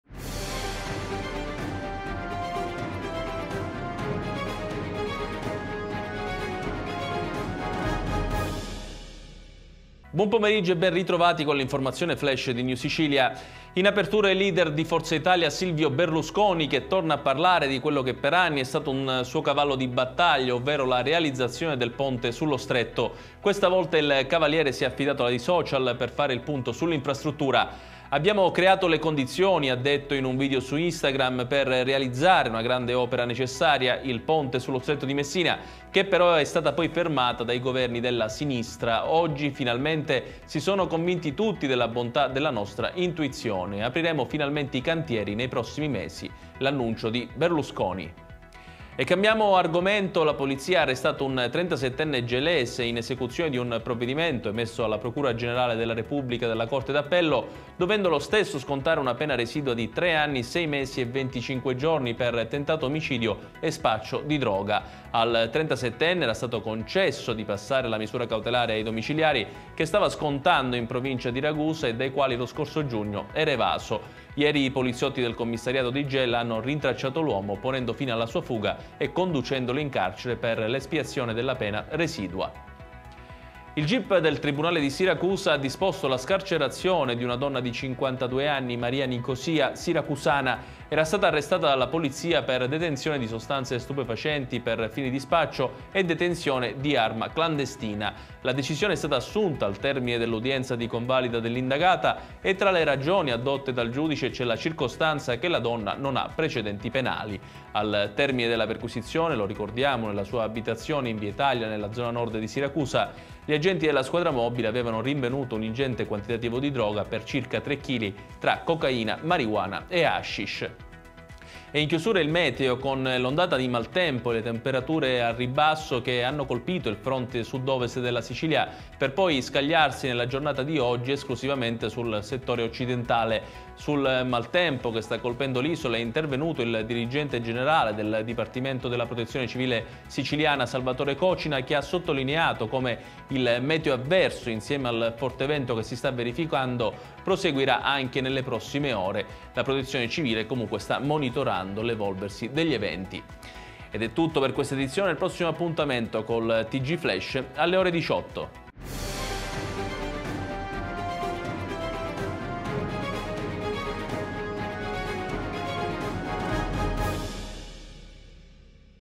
Tg Flash edizione del 21 gennaio delle ore 16,00
Edizione del 21 gennaio 🗓 delle ore 16,00 🕒

• Ponte sullo Stretto, verso la realizzazione. Berlusconi: “Apriremo finalmente i cantieri nei prossimi mesi”➡️

• Tentato omicidio e spaccio di stupefacenti, 37enne irreperibile da 7 mesi arrestato dalla Polizia di Stato➡️

• In casa con 3 chili di droga tra cocaina, marijuana e hashish: scarcerata 52enne➡️

• Maltempo in Sicilia, cosa bisogna aspettarsi? Le parole del capo della Protezione civile Cocina➡️